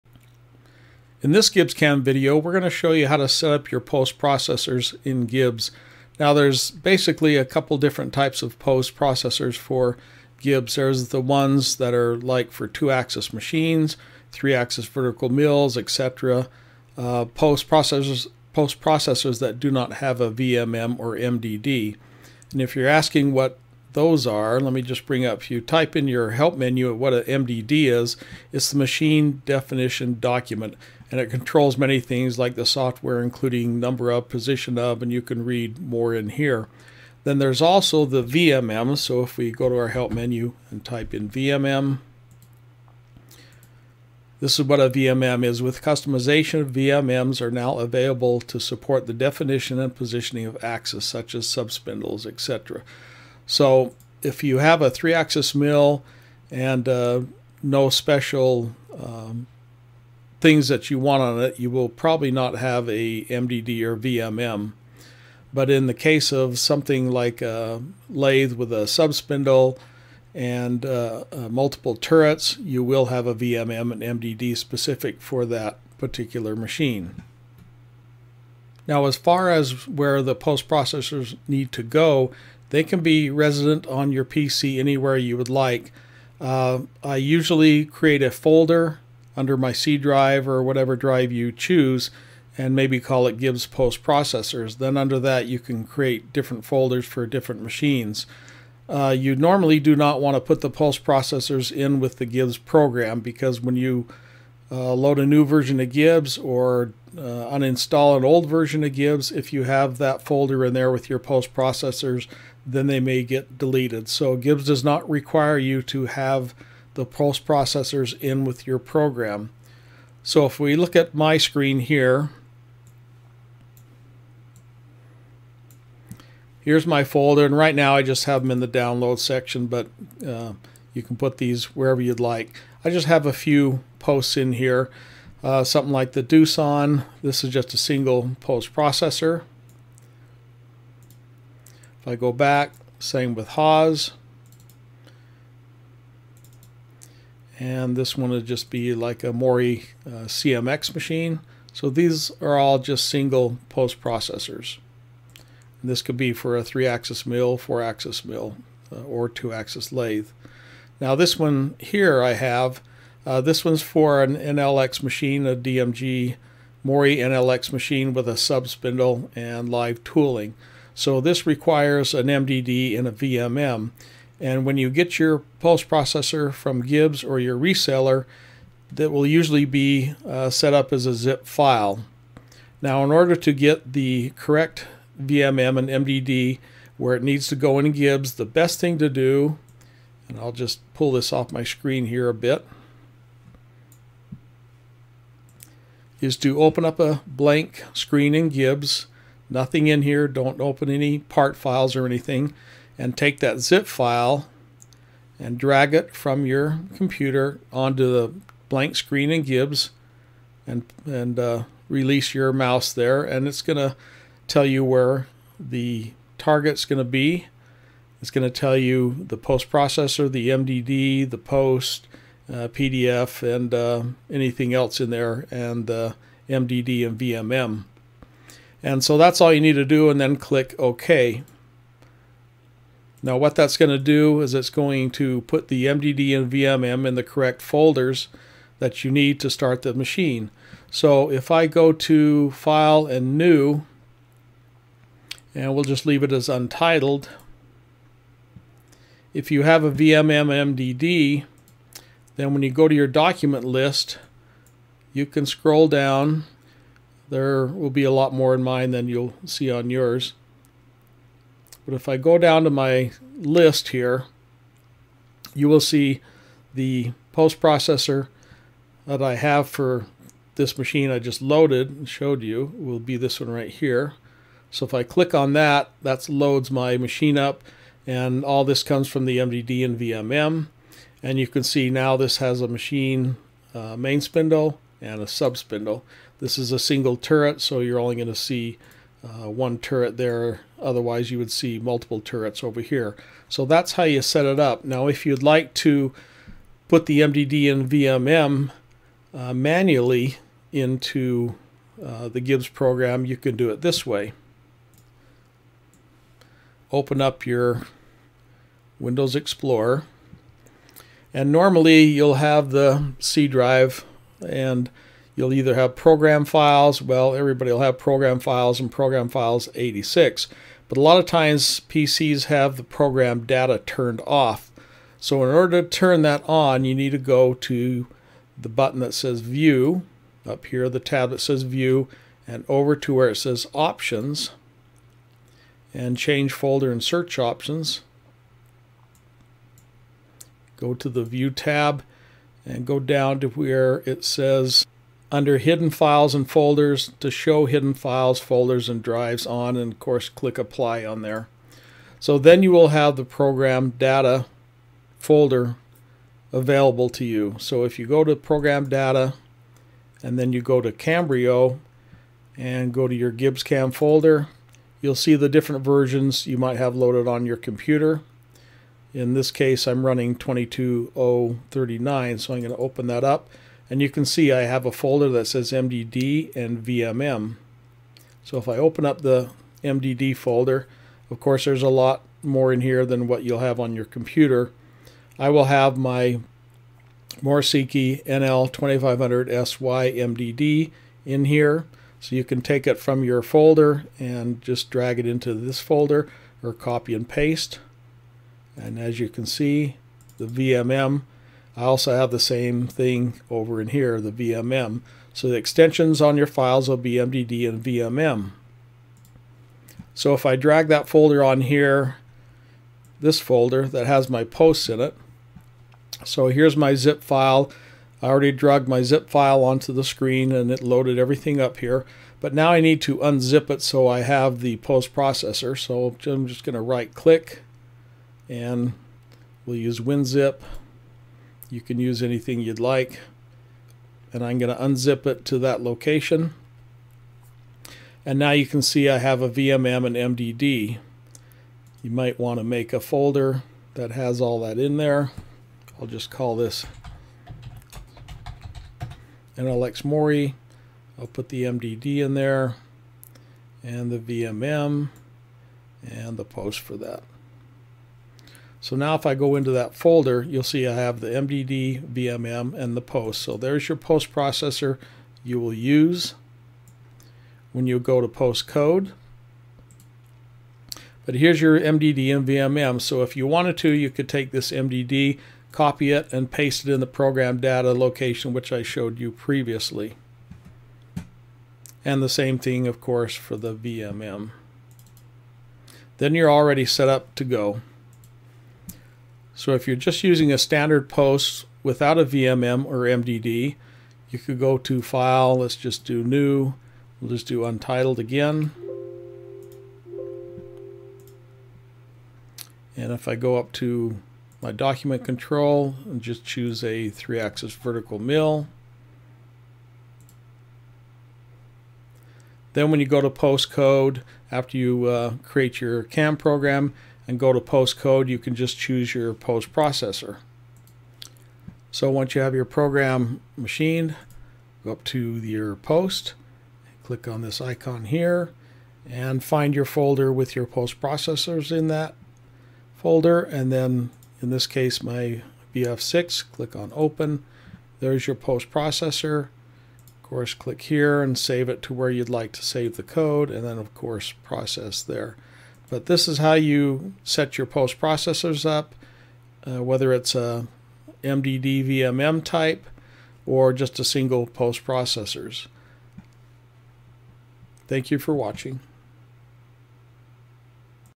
Setting up GibbsCAM Post processors
This video will show you how to set up  your GibbsCAM post processors for your Milling and Turning machine, with or without a MDD and VMM.